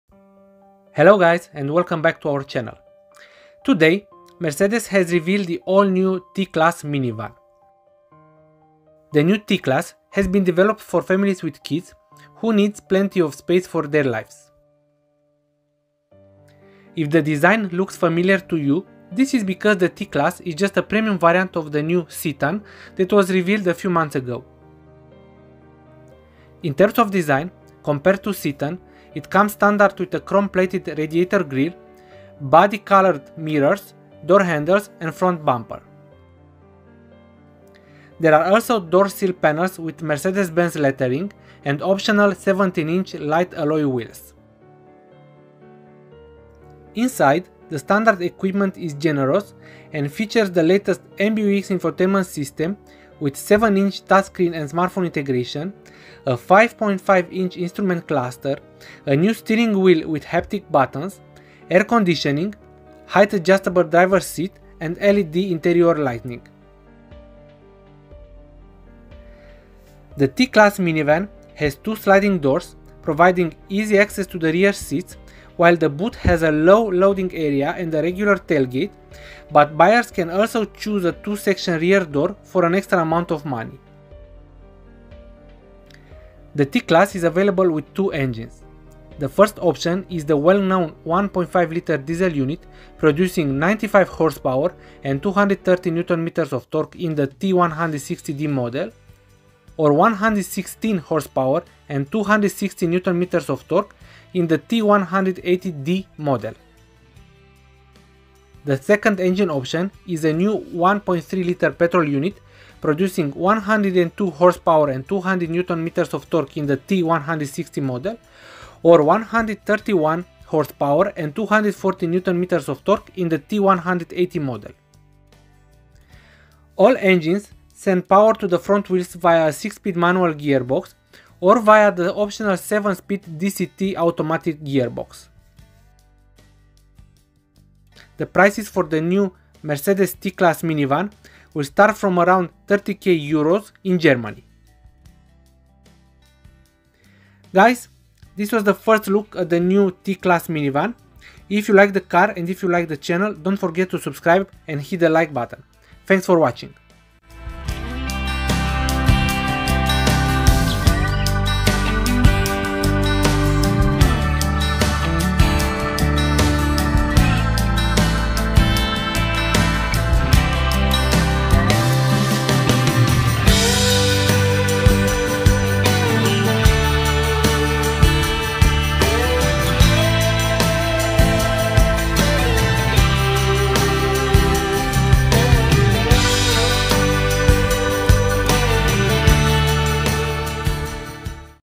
New 2023 Mercedes T-Class: Interior, Exterior presentation
This is a short video review of the new Mercedes T Class 2023. Mercedes T Class 2023 has been developed for families who need plenty of space for their everyday tasks, as well as for weekend trips or sports activities.

Mercedes T Class prices will start from €30,000.

00:00 Exterior
00:47 Interior